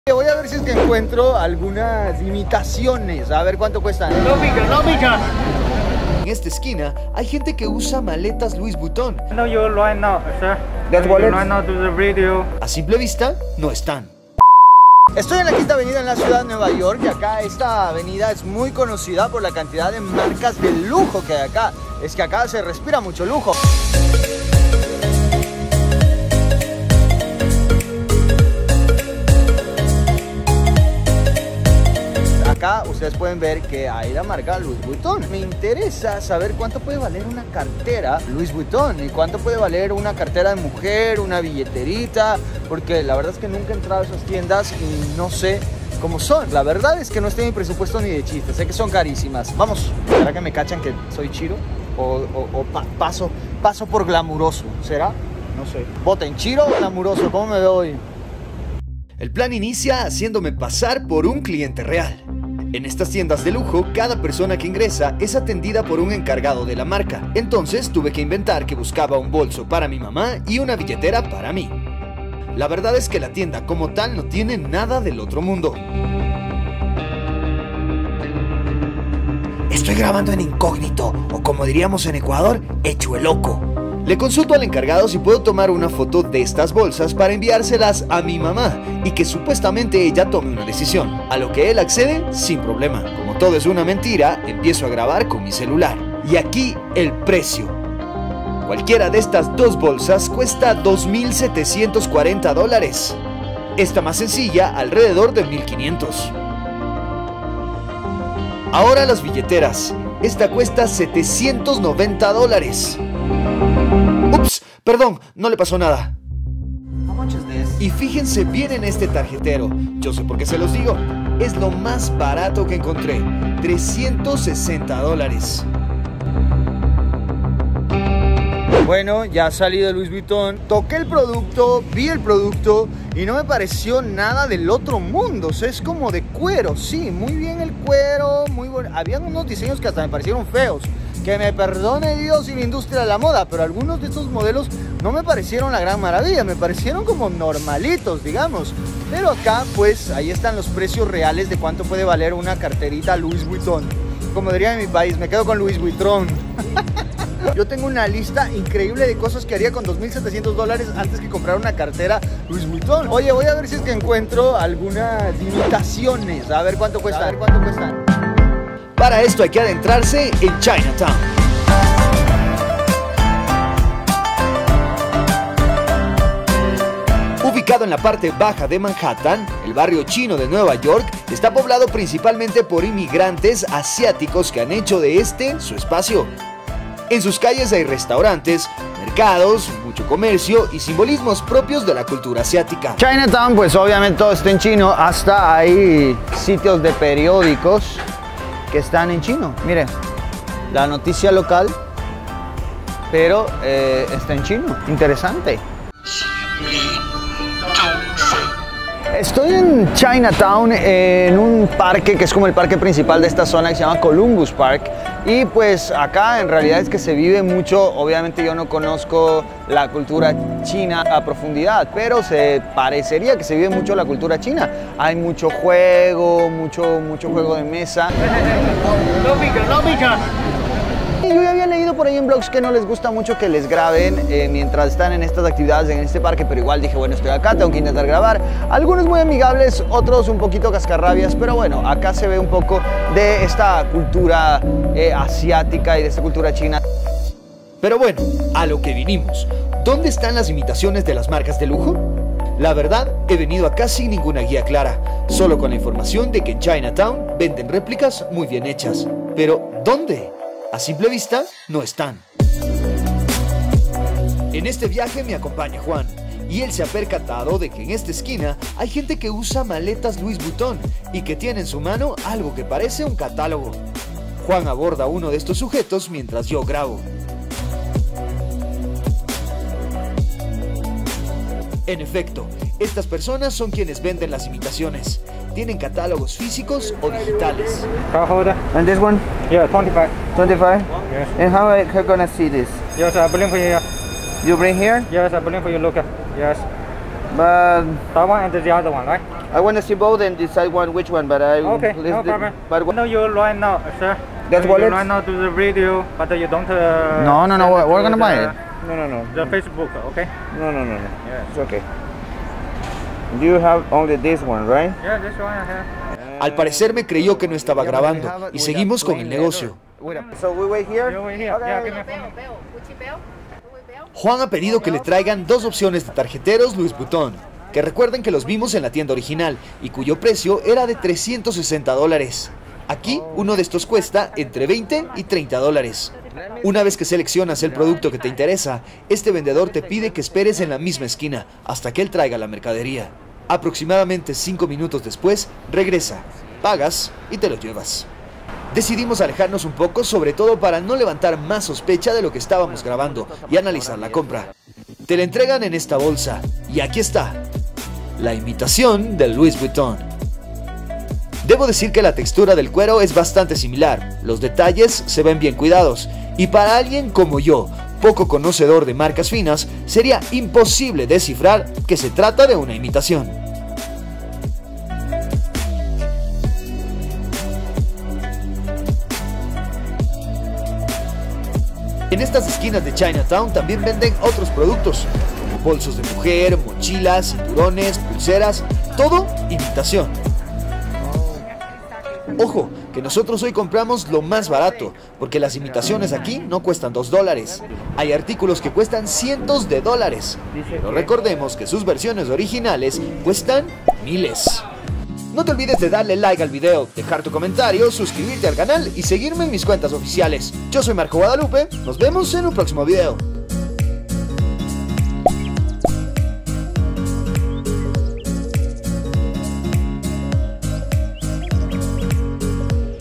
NUEVA YORK| Chinatown - Comprando IMITACIONES 😱
China Town es el famoso sector de Nueva York en el que la comunidad China ha hecho su vida, también es conocido por su excentricidad y por su gran comercio, entre esto, las IMITACIONES DE MARCAS DE LUJO.
¿Cuánto cuesta una cartera Louis Vuitton original? ¿Cuánto cuesta una imitación de Louis Vuitton? Estas preguntas las respondemos en el video.
Descubrimos la forma de comprar imitaciones en China Town, no fue fácil.

00:00 Intro
00:15 Saludo
01:00 Tienda Louis Vuitton
01:40 Precios Louis Vuitton
02:47 Chinatown
03:48 Columbus Park- Chinatown
04:09 Imitaciones Louis Vuitton
04:28 Imitaciones Vendedores
06:00 Original Vs Imitación